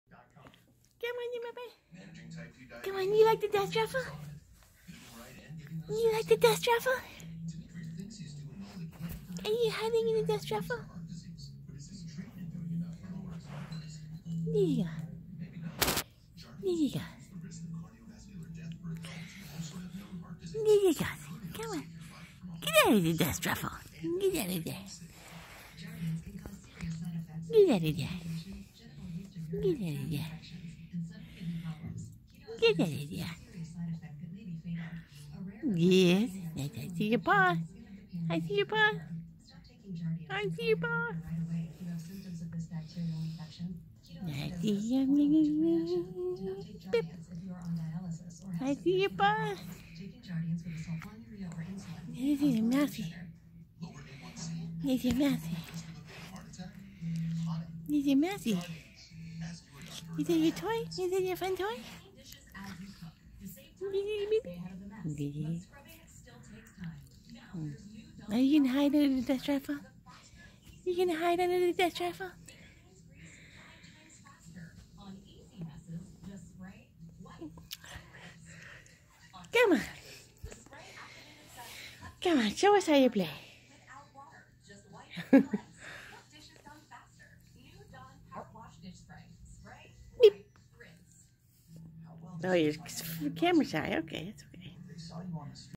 Kitty Templeton and Moo and the Dust Ruffle Feb 2020
Here is Moo hiding in the new Dust Ruffle I put on my bed... February 2020